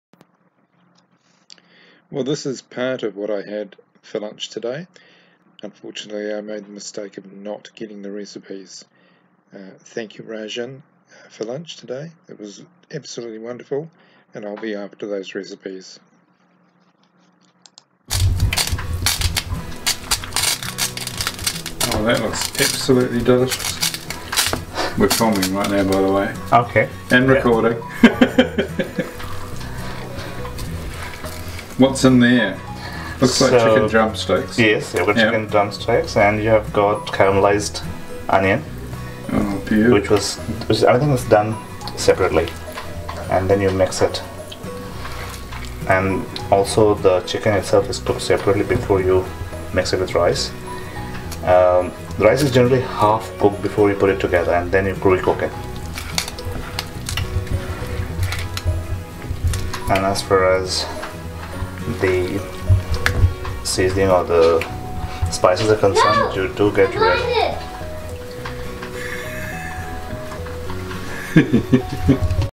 Chicken Biryani and Paneer For Lunch
Part of lunch today with my friend Rajan.  If I can, I will get both these recipes and try to make them.  Nothing like gorgeous food on a summers day.